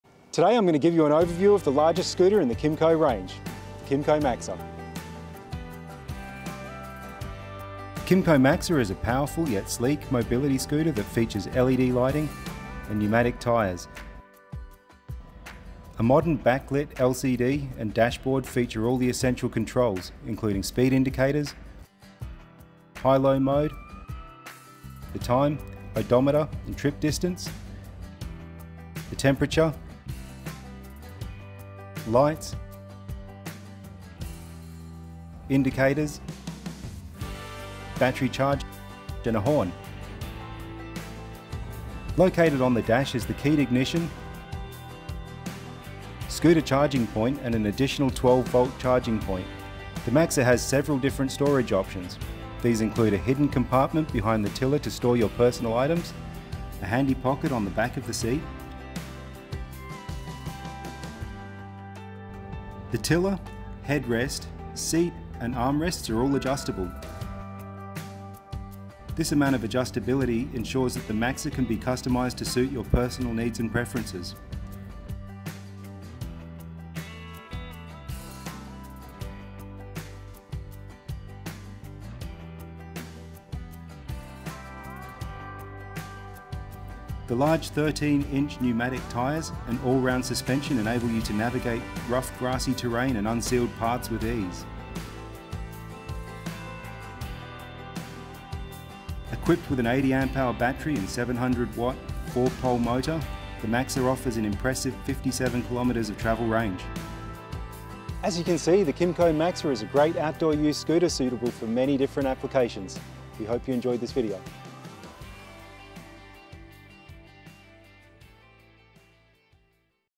Kymco Maxer MOBILITY SCOOTER Overview | Wholesale Mobility
The Maxer is one of the largest scooters in our range and is made to tackle all conditions. Yet, its slimline design will allow you to easily navigate doorways and other entries, while the large 700W 4-pole motor offers enough power to navigate the more challenging terrains safely. The adjustable suspension makes it perfect for most outdoor use, and the 80Ah battery will allow it to travel up to 57km on a single charge. A fully adjustable, contoured captain style seat will provide the ultimate in comfort and versatility.